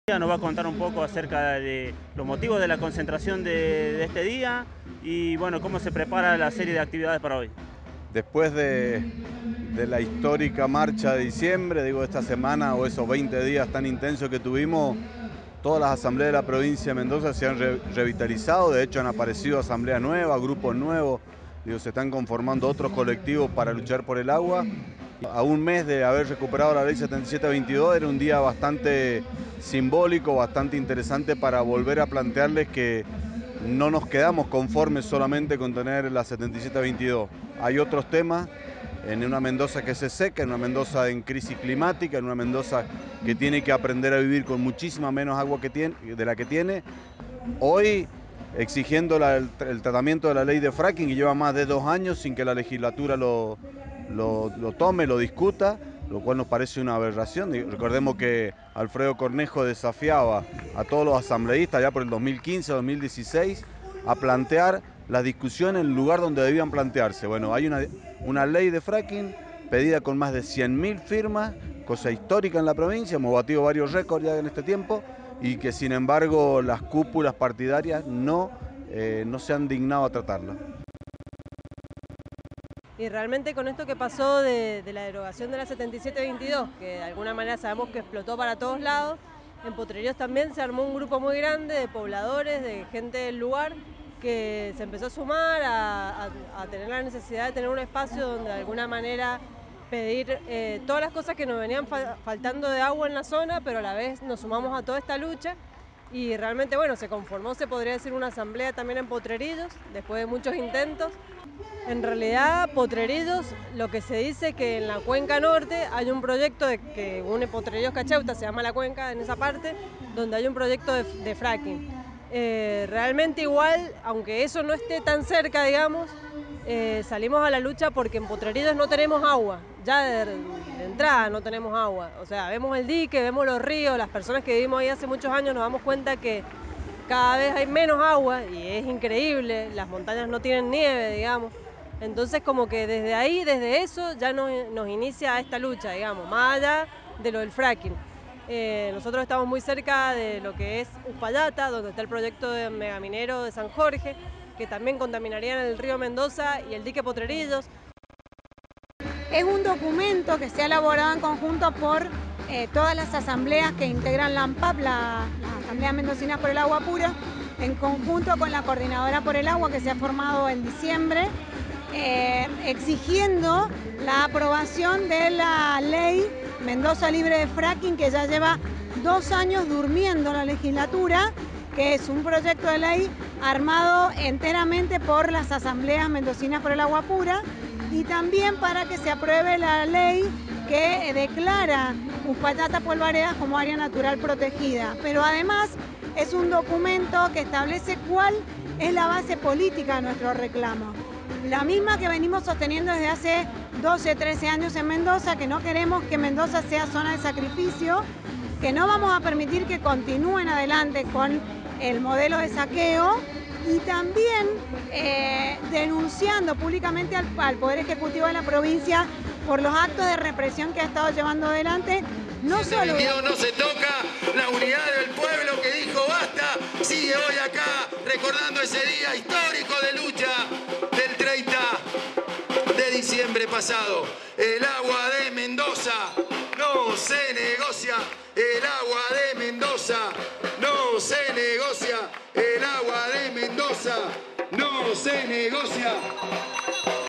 Festejo por la 7722 y exigencia de Ley contra el Fracking
A un mes del histórico triunfo popular en la defensa de la Ley 7722, las Asambleas Socioambientales, organizaron una nueva convocatoria en Defensa del Agua Pura.
En este caso para exigir al gobierno el tratamiento de la Ley contra la explotación con el método de fractura hidráulica en Mendoza.
Hace unos meses que fue acompañada por más de 100 mil firmas, un ejemplo de su trabajo de concientización sobre las consecuencias de este método.
GiraMundo TV Comunitaria acompaño los reclamos, como lo viene haciendo desde hace más de 10 años.